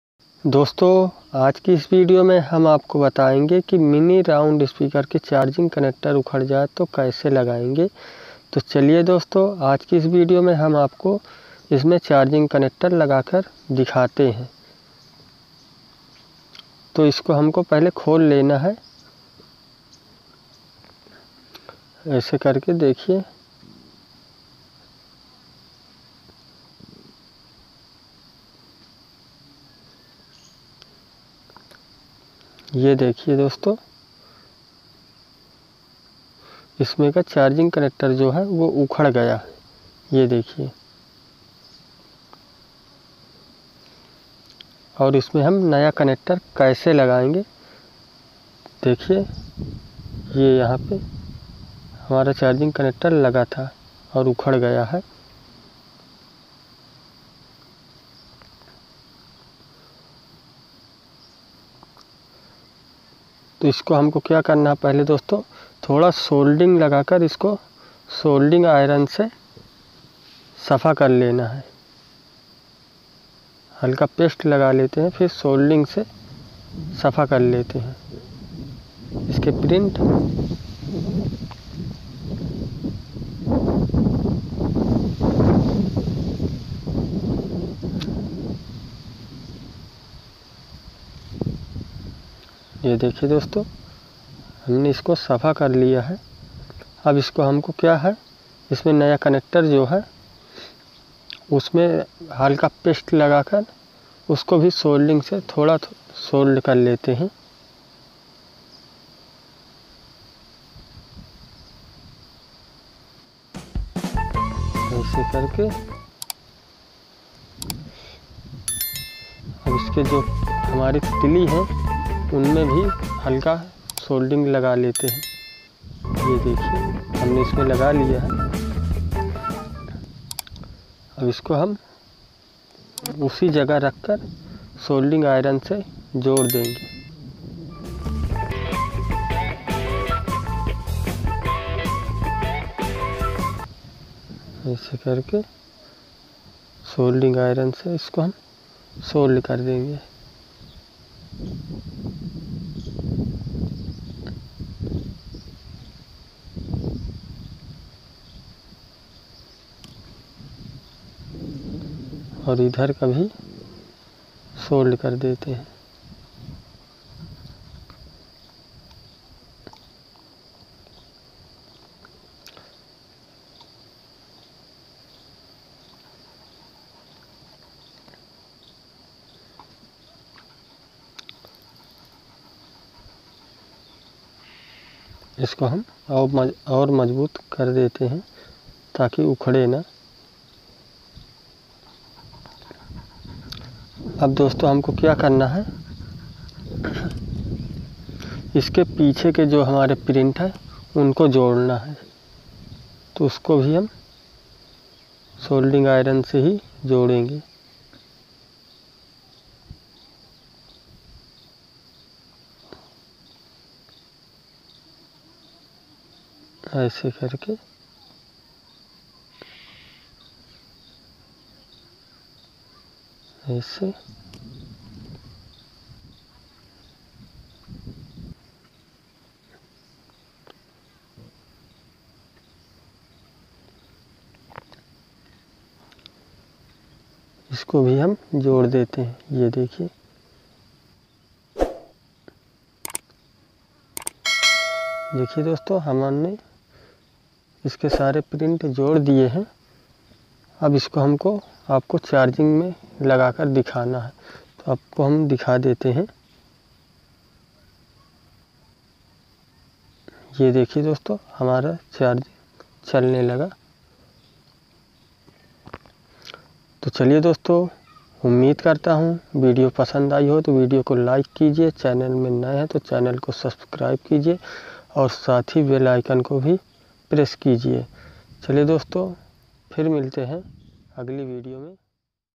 how to repair bluetooth speaker charging port!! speaker repair charging problem Dheerendrasingh
doston aaj ki video mein Ham bataenge ki Mini round speaker Mein charging Jack Kaise Lagate Hain aur lagakar dikhayenge